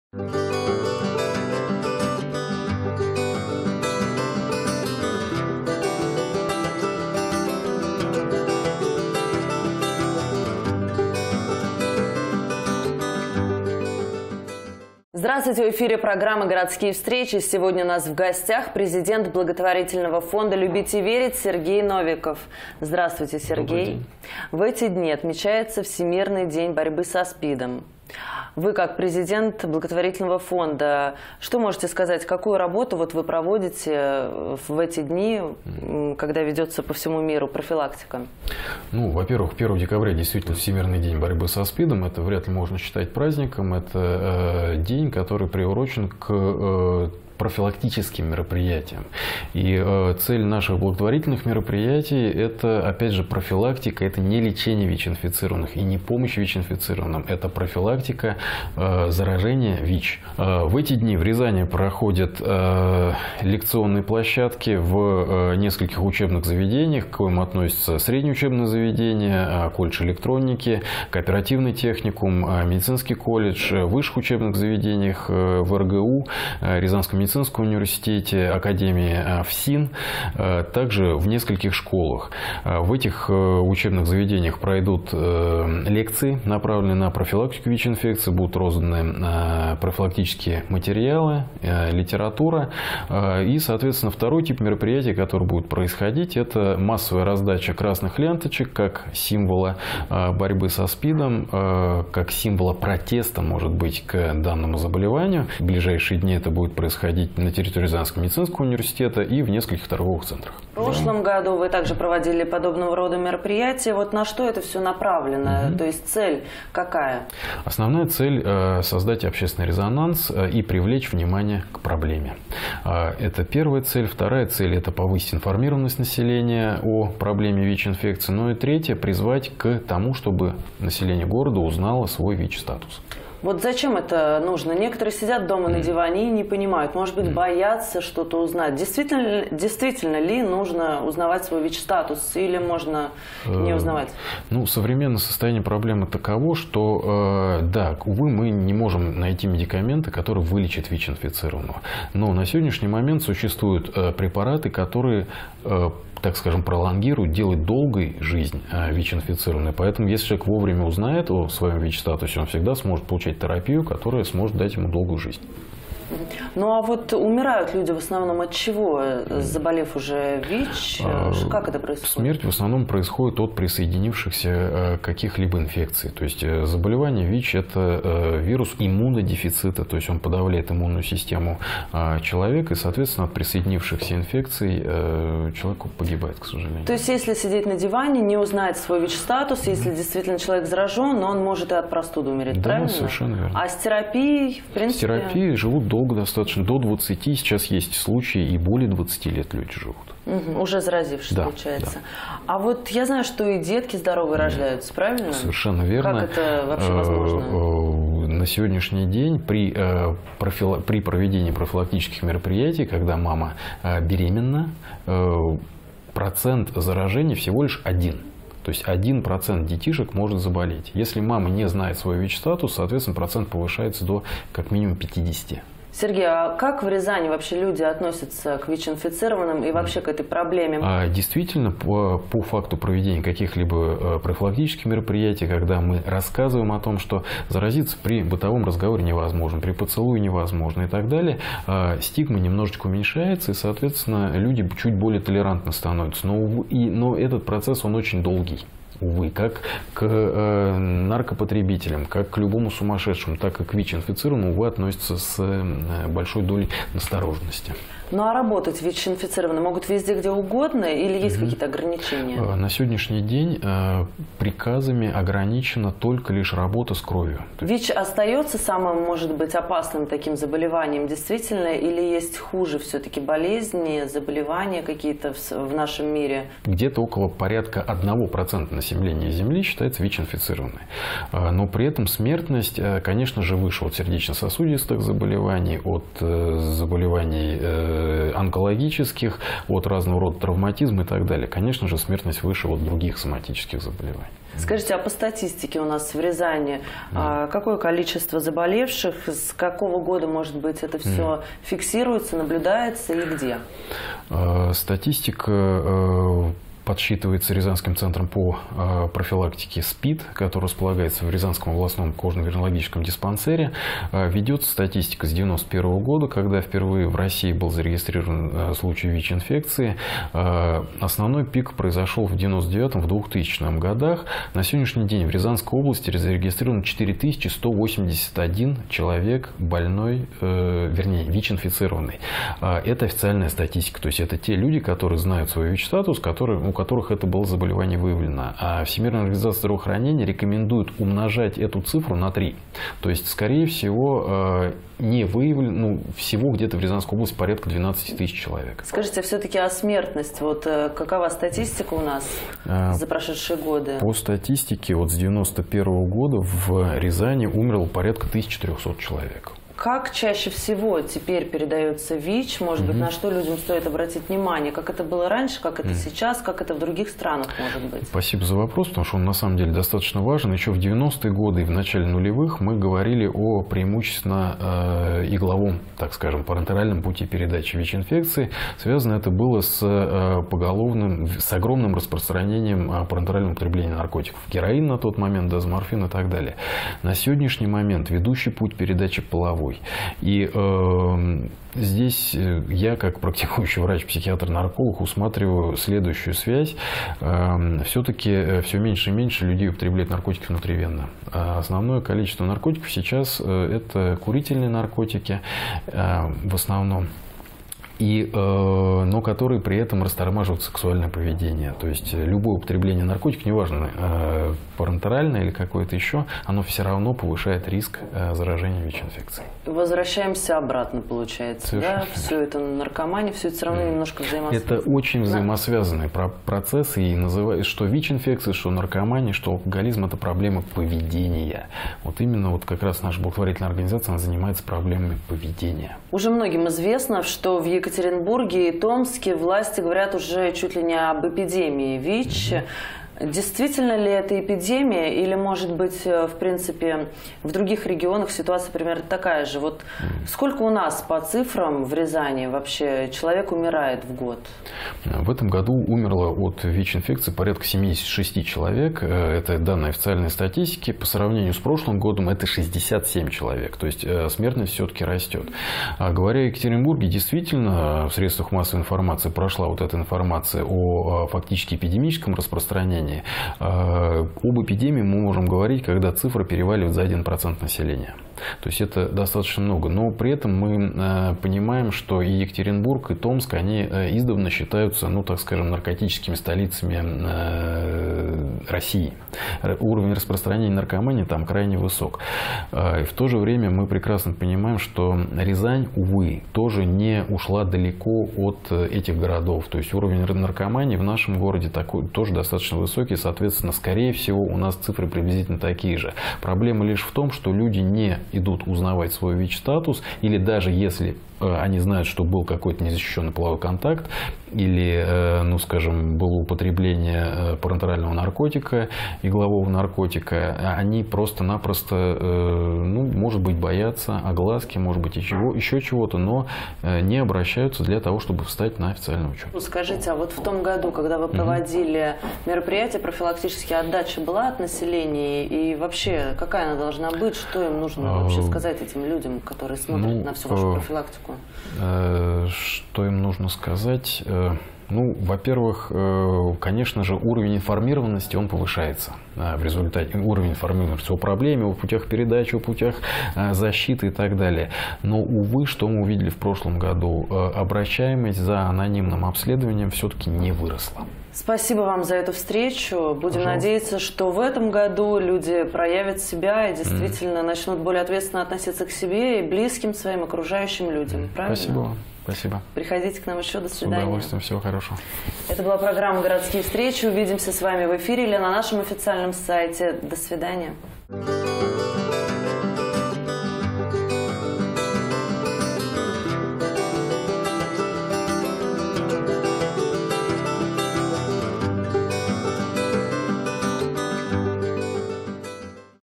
Городские встречи 03.12.16 Сергей Новиков
Президент благотворительного фонда «Любить и верить»